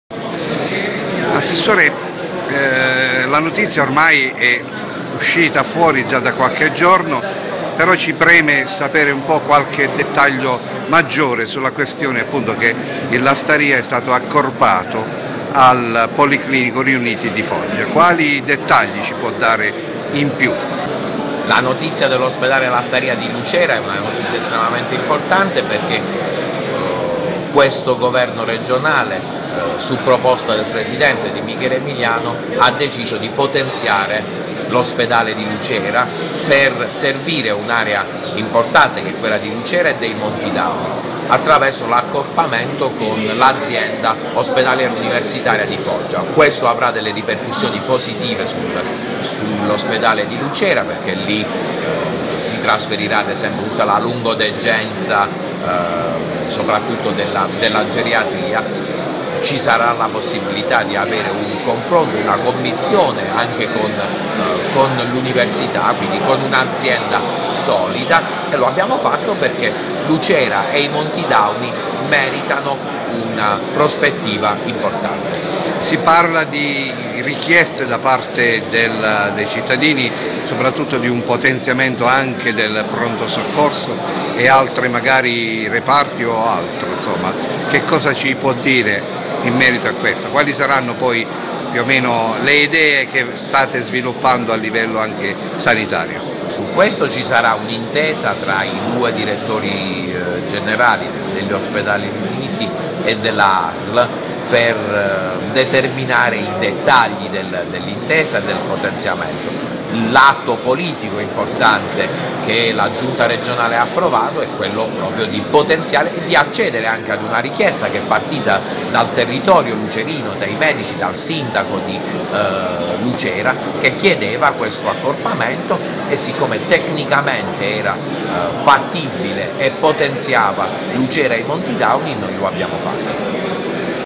Piemontese il Lastaria merita una prospettiva importante studio9tv
Piemontese il Lastaria merita una prospettiva importante

Raffaele Piemontese, assessore al bilancio della Regione Puglia, parla di un Lastaria di Lucera che meritava attenzione maggiore per la rilevanza anche che assume non solo per Lucera ma anche per i Monti Dauni.
Piemontese ha ribadito l'impegno di Emiliano e la sollecitazione ricevuta dai medici e anche da parte del Sindaco, per i dettagli del potenziamento spetterà ai due Direttori Generali, quello dell'Asl e dell'Azienda Ospedaliero.
Piemonte si sbilancia parlando di lungodegenza e geriatria, senza trascurare la collaborazione dell'Università.